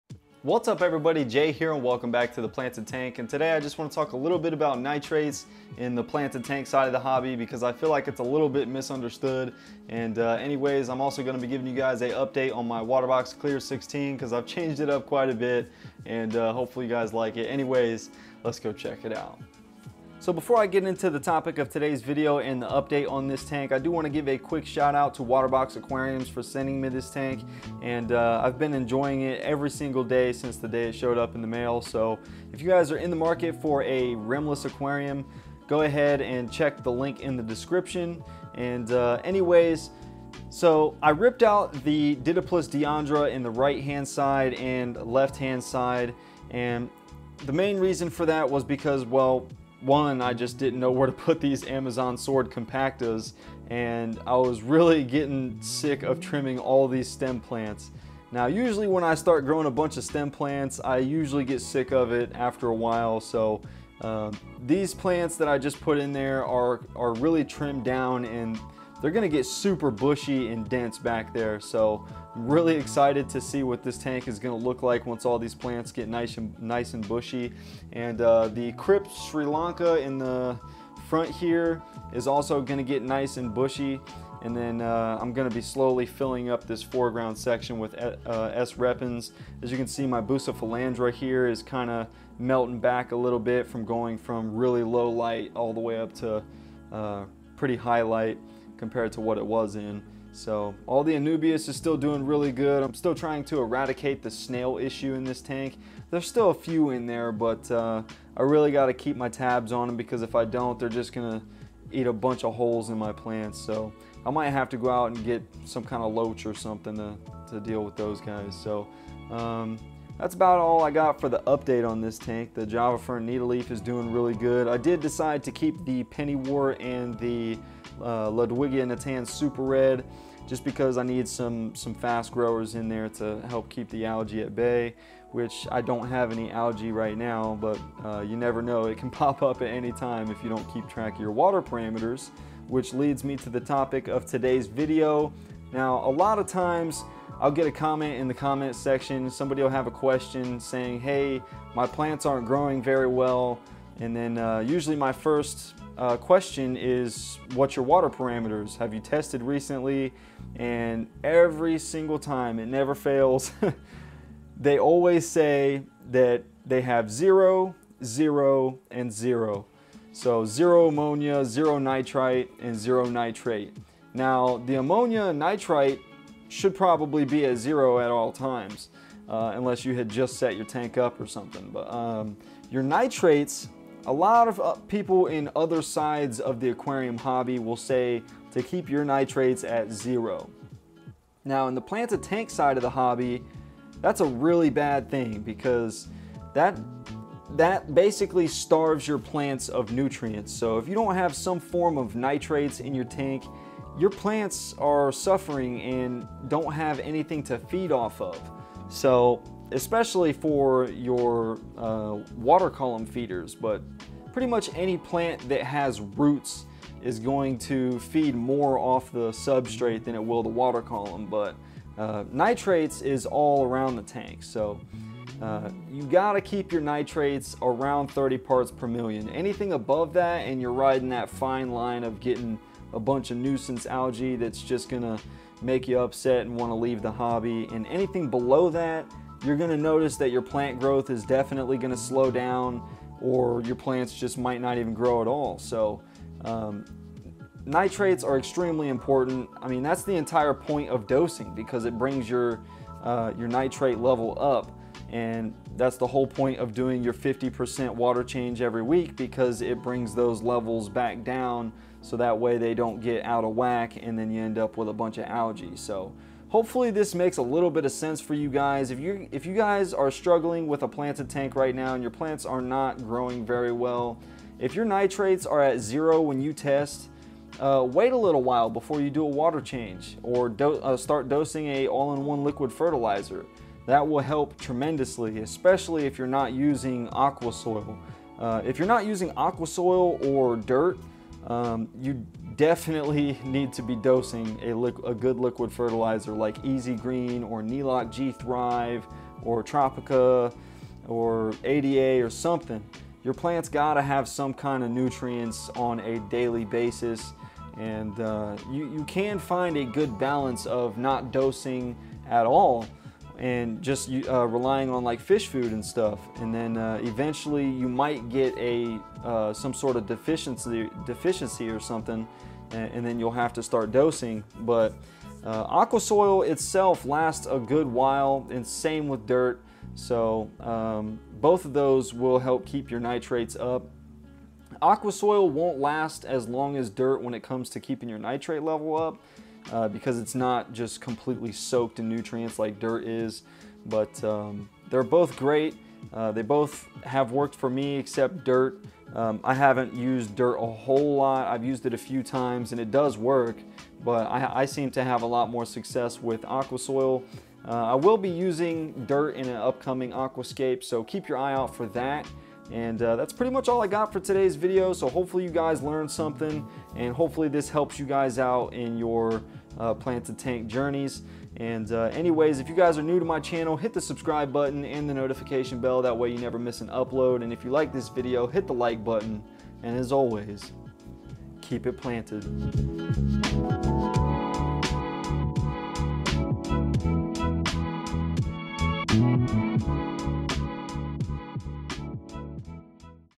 Lets Talk Nitrates
This is a very important part of keeping a planted aquarium healthy long term. If you want success. Keep your nitrates around 30ppm and you will be golden. Test your tanks for nitrates once or twice a month it doesn't hurt anything and only takes 5 minutes of your time!
Please if you are interested in purchasing a rimless aquarium please click the following link.

All things used for this aquarium Below.

The FILTRATION is a Oase Biomaster Thermo 250
I plan on switching all of my filters to Oase filters soon. They are simply put. The Best!

The LIGHTING ( 9hrs a Day ) on this tank is a Twinstar 600e
Thing really grows plants and makes everything pop with color!

CO2
Using a AquTek Mini Paintball CO2 regulator! Works great for cheap!
I also use this cheap inline CO2 diffuser and it works great!

Substrate being used it UNS Controsoil Fine Type

I also use Seachem Flourish Tabs for my heavy Root feeders

I use tap water in this aquarium for water changes and dechlorinate with Seachem Prime

I use NilocG Thrive+ to dose!

Amazon sword compacta
Cryptocoryne Spiralis
Cryptocoryne Sri Lanka
Staurogyne Repens
Java Fern Needle Leaf
Anubias Nan Petitie
Anubias Laneolata
Bucephalandra Wavy Green
Ludwigia Natans Super Red
Brazilian Pennywort